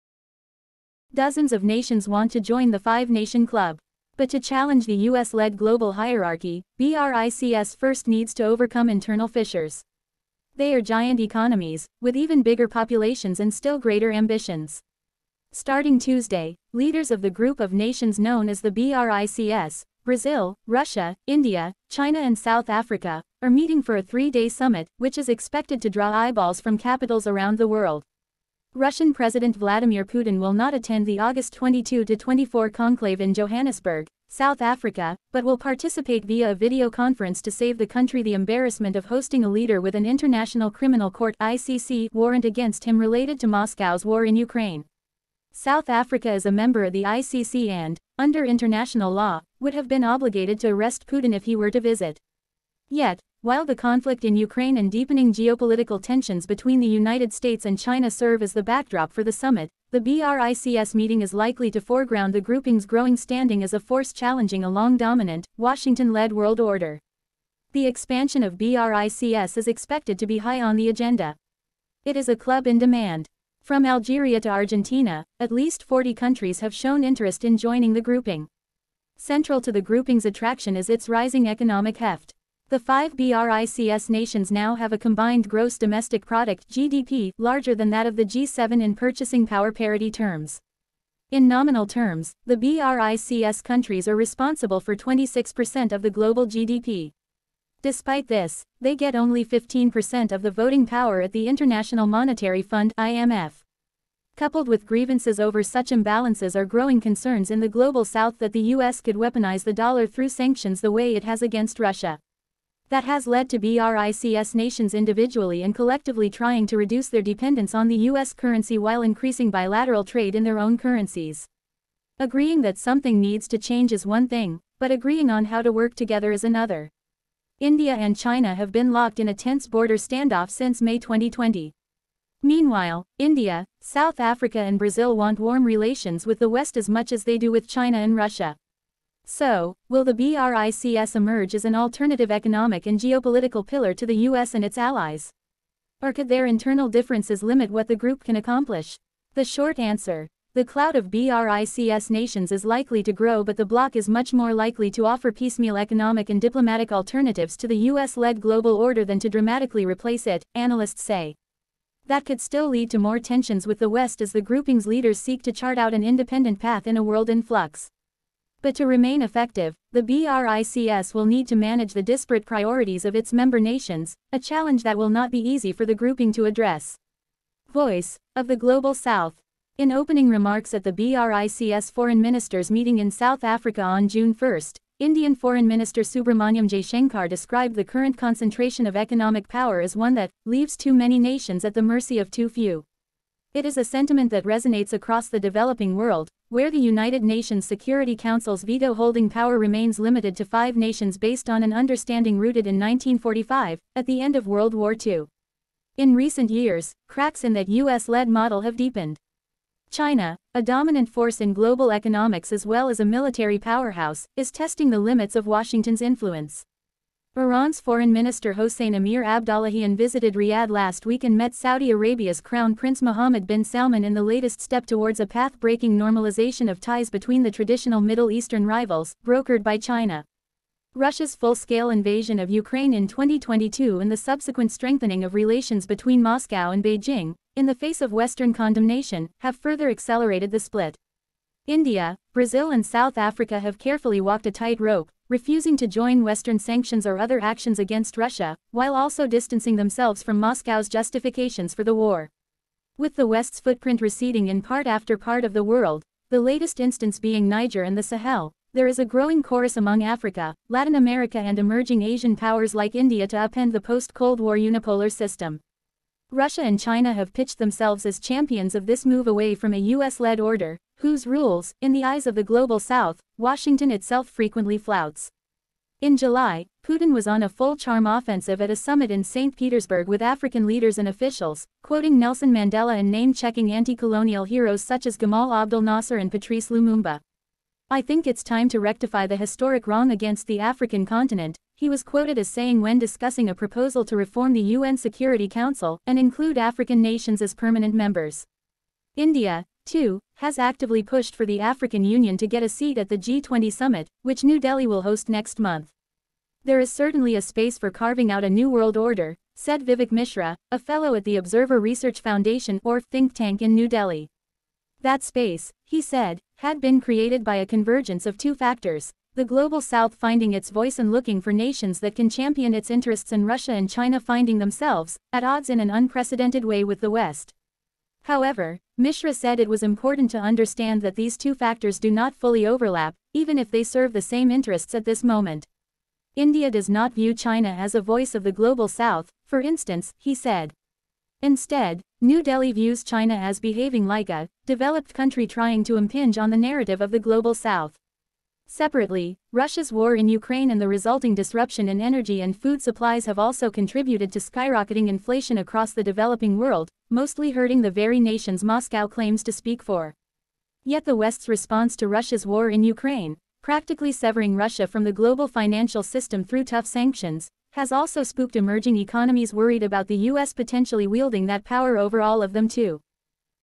Can BRICS create a new world order? - W&R News
Dozens of nations want to join the five-nation club. But to challenge the US-led global hierarchy, BRICS first needs to overcome internal fissures

They are giant economies, with even bigger populations and still greater ambitions. Starting Tuesday, leaders of the group of nations known as the BRICS – Brazil, Russia, India, China and South Africa – are meeting for a three-day summit, which is expected to draw eyeballs from capitals around the world.

Russian President Vladimir Putin will not attend the August 22-24 conclave in Johannesburg, South Africa, but will participate via a video conference to save the country the embarrassment of hosting a leader with an International Criminal Court (ICC) warrant against him related to Moscow’s war in Ukraine. South Africa is a member of the ICC and, under international law, would have been obligated to arrest Putin if he were to visit.

Yet, while the conflict in Ukraine and deepening geopolitical tensions between the United States and China serve as the backdrop for the summit, the BRICS meeting is likely to foreground the grouping’s growing standing as a force challenging a long-dominant, Washington-led world order.

The expansion of BRICS is expected to be high on the agenda. It is a club in demand. From Algeria to Argentina, at least 40 countries have shown interest in joining the grouping.

Central to the grouping’s attraction is its rising economic heft. The five BRICS nations now have a combined gross domestic product (GDP) larger than that of the G7 in purchasing power parity terms. In nominal terms, the BRICS countries are responsible for 26 percent of the global GDP. Despite this, they get only 15 percent of the voting power at the International Monetary Fund (IMF).

Coupled with grievances over such imbalances are growing concerns in the Global South that the US could weaponise the dollar through sanctions the way it has against Russia. That has led to BRICS nations individually and collectively trying to reduce their dependence on the US currency while increasing bilateral trade in their own currencies.

Agreeing that something needs to change is one thing, but agreeing on how to work together is another. India and China have been locked in a tense border standoff since May 2020. Meanwhile, India, South Africa and Brazil want warm relations with the West as much as they do with China and Russia.

So, will the BRICS emerge as an alternative economic and geopolitical pillar to the US and its allies? Or could their internal differences limit what the group can accomplish?

The short answer: The clout of BRICS nations is likely to grow but the bloc is much more likely to offer piecemeal economic and diplomatic alternatives to the US-led global order than to dramatically replace it, analysts say. That could still lead to more tensions with the West as the grouping’s leaders seek to chart out an independent path in a world in flux. But to remain effective, the BRICS will need to manage the disparate priorities of its member nations – a challenge that will not be easy for the grouping to address.

‘Voice’ of the Global South
In opening remarks at the BRICS foreign ministers meeting in South Africa on June 1, Indian Foreign Minister Subrahmanyam Jaishankar described the current concentration of economic power as one that “leaves too many nations at the mercy of too few”.

It is a sentiment that resonates across the developing world, where the United Nations Security Council’s veto-holding power remains limited to five nations based on an understanding rooted in 1945, at the end of World War II.

In recent years, cracks in that US-led model have deepened. China, a dominant force in global economics as well as a military powerhouse, is testing the limits of Washington’s influence. Iran’s Foreign Minister Hossein Amir-Abdollahian visited Riyadh last week and met Saudi Arabia’s Crown Prince Mohammed bin Salman in the latest step towards a pathbreaking normalisation of ties between the traditional Middle Eastern rivals, brokered by China.

Russia’s full-scale invasion of Ukraine in 2022 and the subsequent strengthening of relations between Moscow and Beijing – in the face of Western condemnation – have further accelerated the split. India, Brazil and South Africa have carefully walked a tight-rope, refusing to join Western sanctions or other actions against Russia, while also distancing themselves from Moscow’s justifications for the war.

With the West’s footprint receding in part after part of the world – the latest instance being Niger and the Sahel – there is a growing chorus among Africa, Latin America and emerging Asian powers like India to upend the post-Cold War unipolar system.

Russia and China have pitched themselves as champions of this move away from a US-led order, whose rules – in the eyes of the Global South – Washington itself frequently flouts.